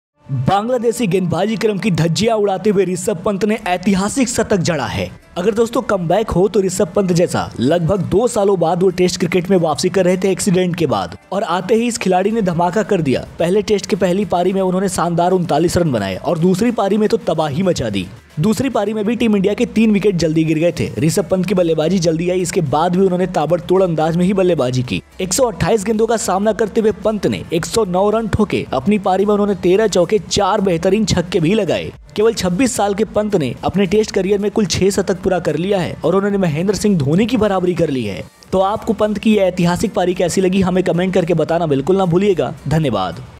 Watch rishabh pant 109 full inning vs Bangladesh| pant century | IND vs Ban 1st test
Cricket Expert TEAM.